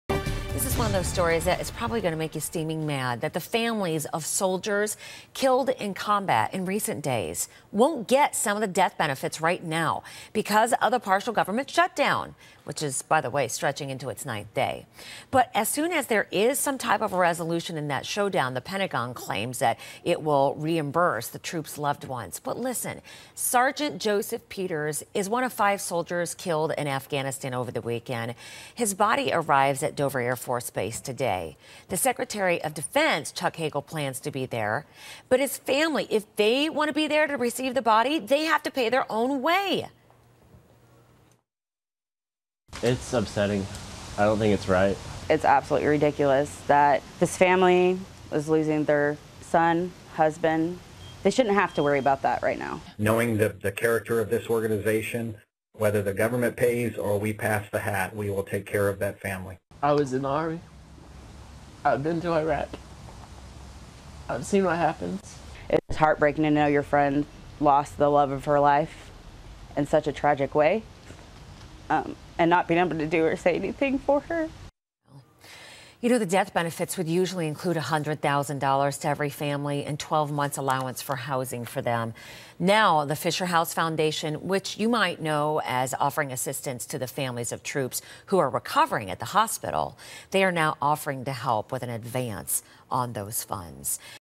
On shutdowns 9th day survivors of fallen troops take center stage
When Defense Secretary Chuck Hagel visits Dover Air Force Base Wednesday, he won't just be honoring troops killed in Afghanistan. His presence will also send a message about the government shutdown.

Hagel will attend a ceremony for the return of soldiers' remains -- at a time the families of those service members are not receiving the standard benefits typically given to them.

Due to the partial federal government shutdown, those benefits, which include a $100,000 payment, are being withheld.

It's the "worst nightmare" for military families, says Amy Neiberger-Miller of TAPS, a support network for families of the fallen.